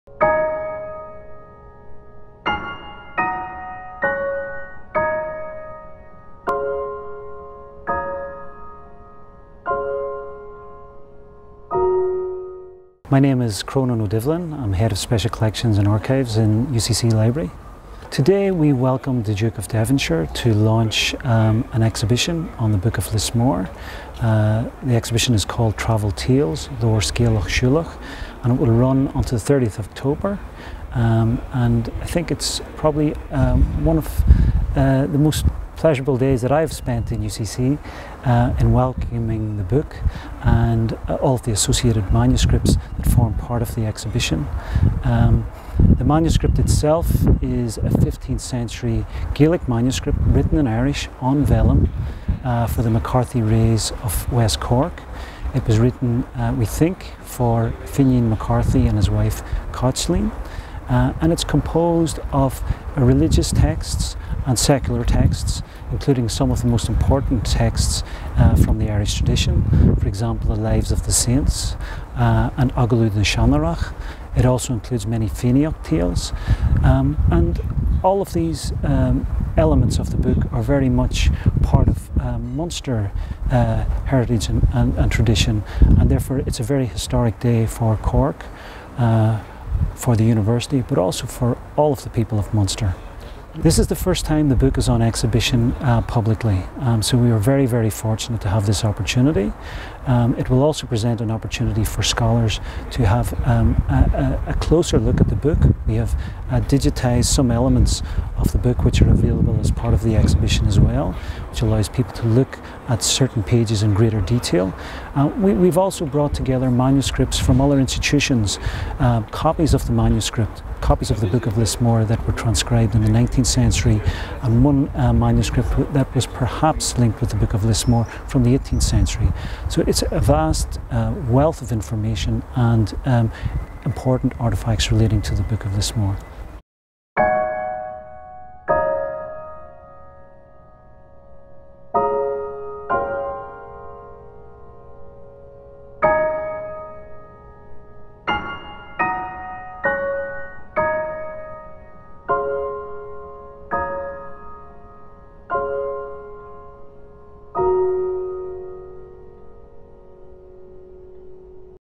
The Book of Lismore at the Glucksman Gallery, UCC
University College Cork (UCC) has gained a unique opportunity
to exhibit on loan one of the major medieval Irish manuscripts,
the Book of Lismore. The loan has been facilitated by the owners
of the Book, the Trustees of the Chatsworth Settlement Trust.  The
Exhibition was officially launched by the Duke of Devonshire at the Lewis
Glucksman Gallery, UCC and will be on public display until 30 October at the
Gallery in an exhibition titled: Travelled Tales - Leabhar Scéalach Siúlach:
The Book of Lismore at University College Cork.  The Glucksman Gallery is
open Tue-Sat 10am-5pm; Sun 2-5pm.

Crónán Ó Doibhlin, Head of Special Collections, Archives and Repository
Services, UCC Library

Researcher:  Marie McSweeney, Office of Media & Communications, UCC